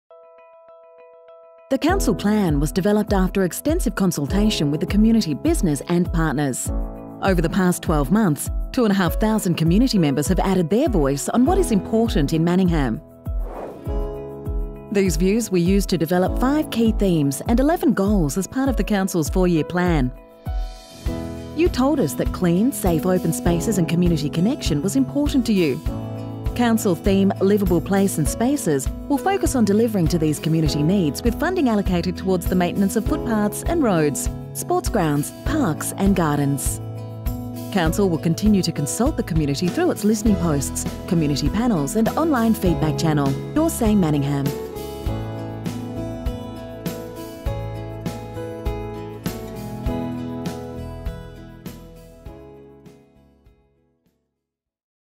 Council Plan: Liveable Places and Spaces
LIVEABLE PLACES AND SPACES

From leafy large blocks in the outer suburbs, past a green wedge, and through to newly evolving high density communities in Doncaster.

It is important that parks, open spaces and streetscapes are maintained to provide people with space to relax, exercise and travel easily from place to place.

Council will focus on managing amenity to create inviting places and spaces, enhanced parks, open space and streetscapes, well connected, safe and accessible travel and well utilised and maintained community infrastructure.

Goals:
- Inviting places and spaces
- Enhanced parks, open space and streetscapes
- Well connected, safe and accessible travel
- Well utilised and maintained community infrastructure

ABOUT THE COUNCIL PLAN

Over 2500 of our local community added their voice on what’s important in Manningham. The Council Plan was developed following extensive consultation involving the community, business, partners, Councillors and Council staff.

Vision:
A liveable and harmonious City

Mission:
A financially sustainable Council that listens, consults and acts with integrity, value and transparency